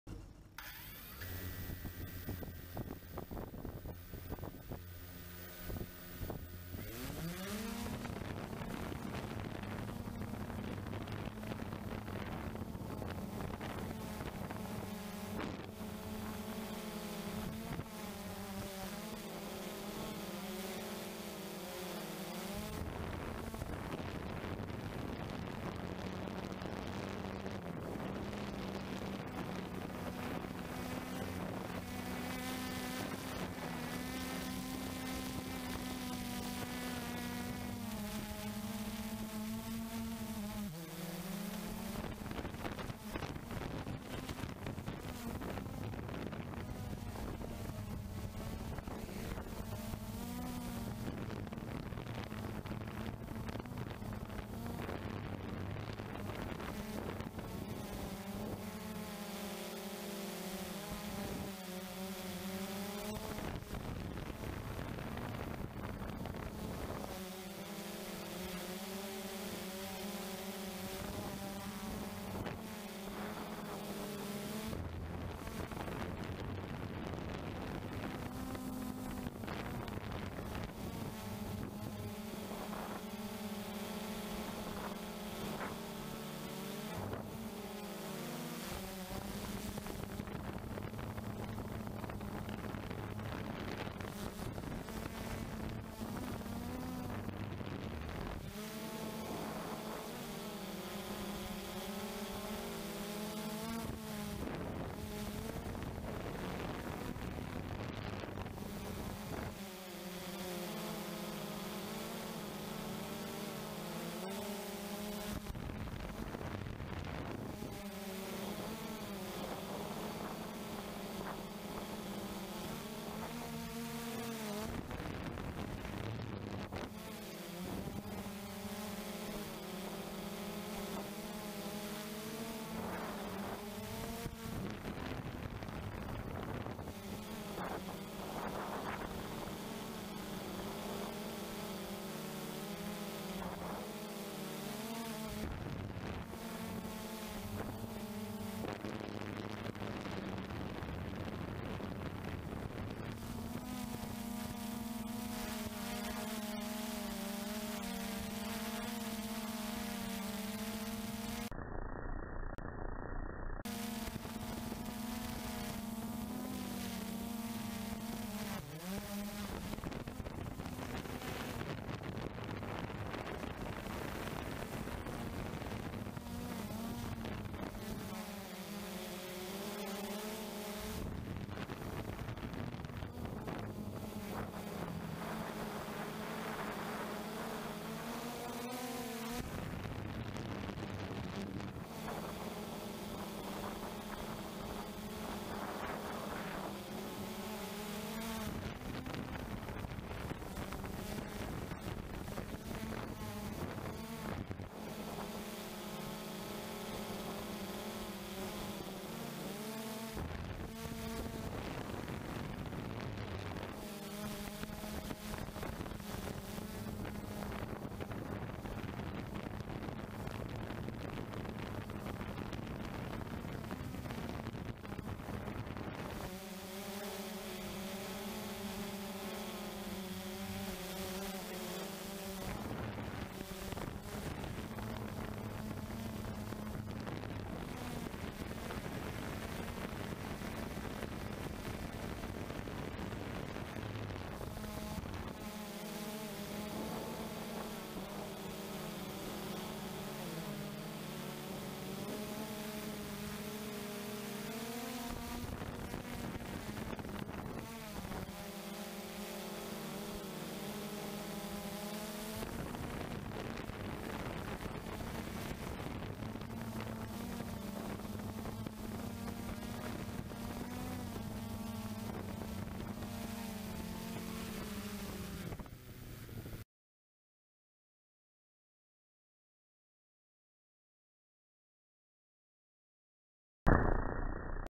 Near mid-air collision... with a Bee!?!
On a routine RTH a Bee flew thru our rotor-wash and was caught on our video.

Specs:
Blade 350 QX3
GB200 Brushless Gimbal
Eflite 3000mah 3S Lipo
Stock "B" Props
GoPro 3+ Black
Immersion RC EZ-OSD
Immersion RC 600mw VTX